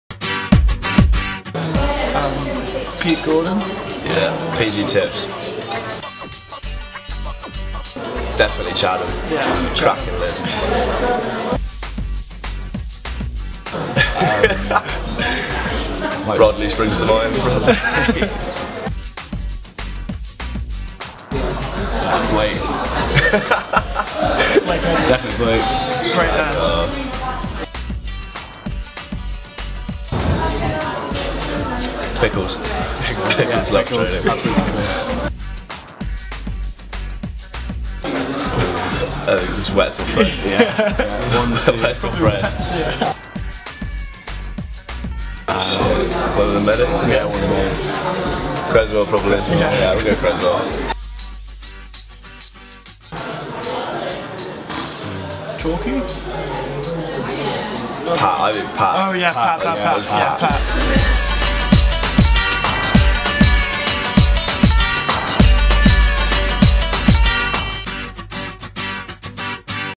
VARSITY TEAM MATES - Rugby Team
We interviewed Cameron, Ali and Ollie for their upcoming Varsity Rugby match.

Video by: James Bachmann, Jack Caton